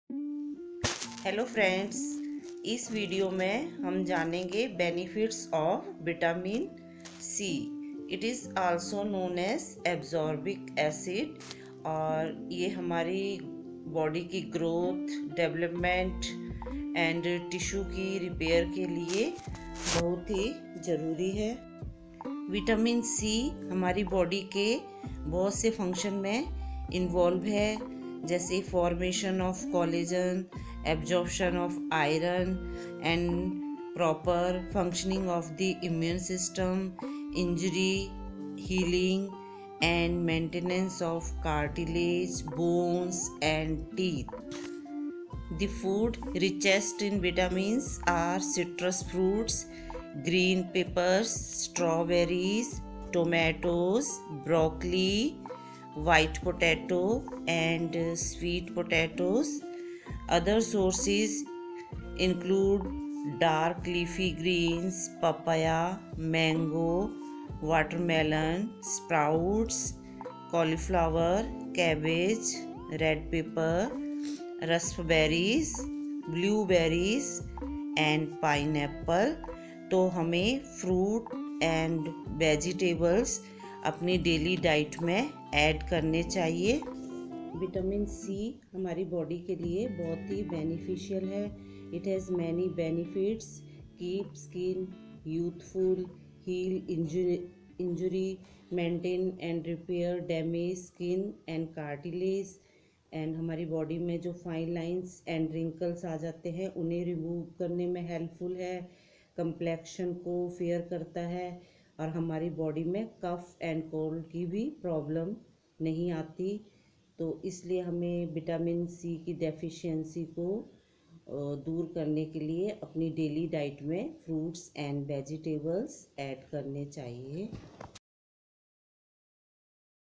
HOW TO REMOVE VITAMIN C DEFICIENCY, BENEFITS OF VITAMIN C#vegetables#fruits#vitaminC#Shorts#
Benefits of vitamin c..It is also known as absorbic acid.Necessary for the growth, development n repair of all body tissue.It involved in many functions including formation of collagen, absorption of iron the proper functioning of the immune system.It is found in strawberries, tomatoesbroccoli,sweer potatoes,mango,papaya, watermelon, cauliflower,sprouts etc1.keep skin youthful2. maintain n repair damaged skin n cartilage 3.diminish the appearance of the fine lines wrinkles 4.even skin tone 5. prevent premature ageing.